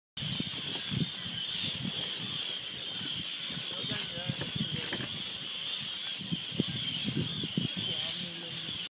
Iceland Whale Watching, 17
Aboard the schooner HauKur, of North Sailing out of Husavik.